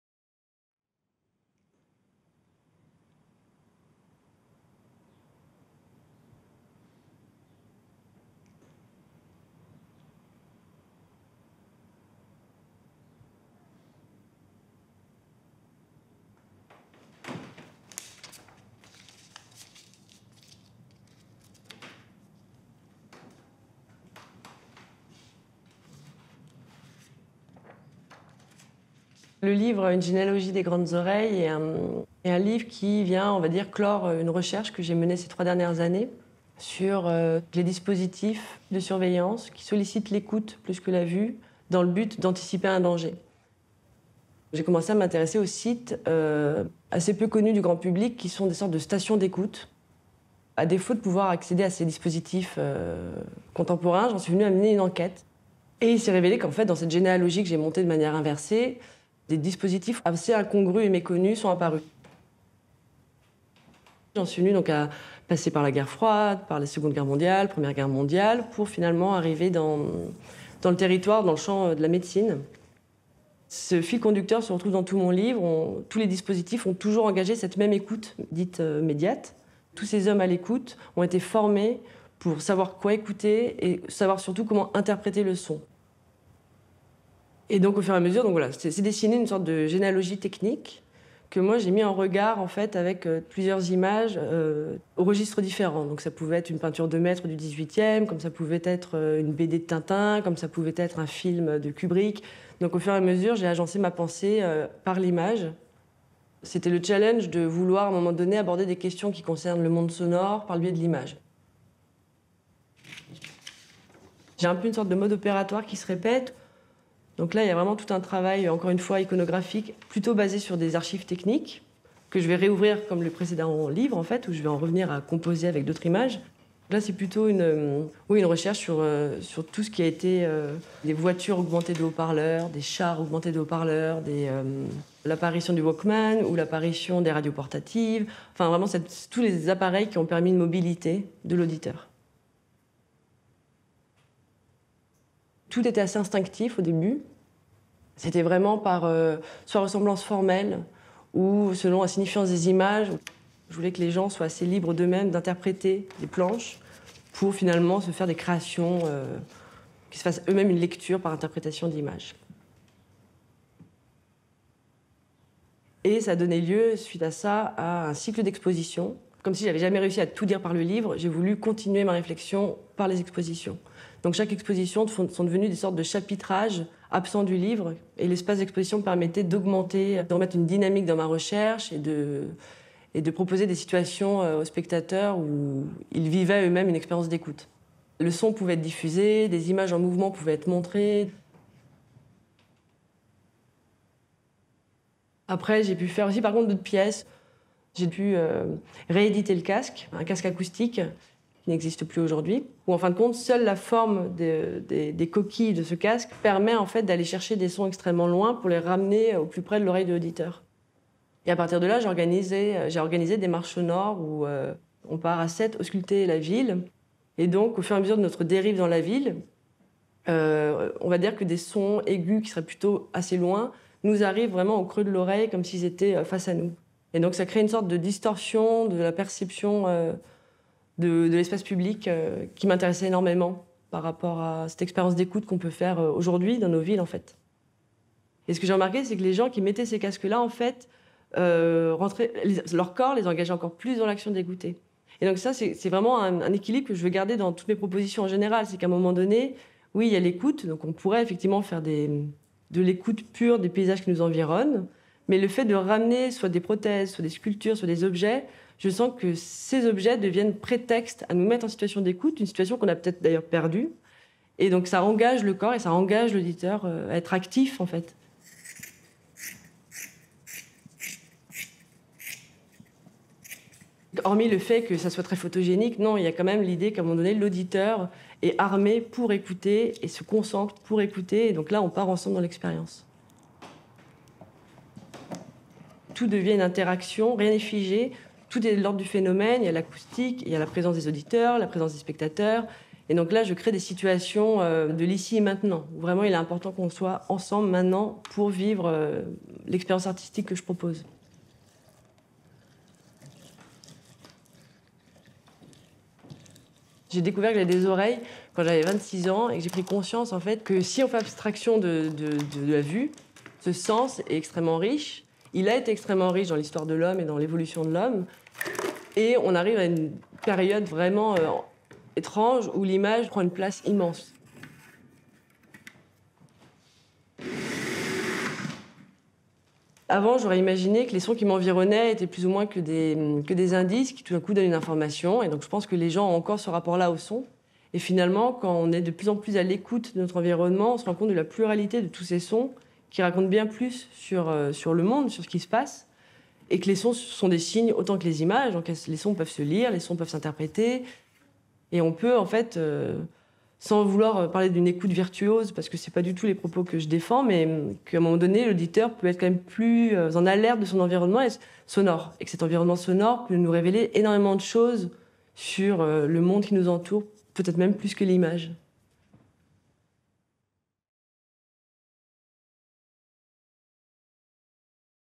Lauren TORTIL - Révélation Livre d'artiste 2020 - Atelier A / ADAGP
Influencée par les « Sound Studies » et l’archéologie des médias, Lauren Tortil s’interroge sur l’histoire des technologies sonores contemporaines et le rapport que l’Homme entretient avec l’écoute. Son ouvrage Une généalogie des grandes oreilles présente sa dernière recherche iconographique au sujet des dispositifs de surveillance qui sollicitent l’écoute plus que la vue.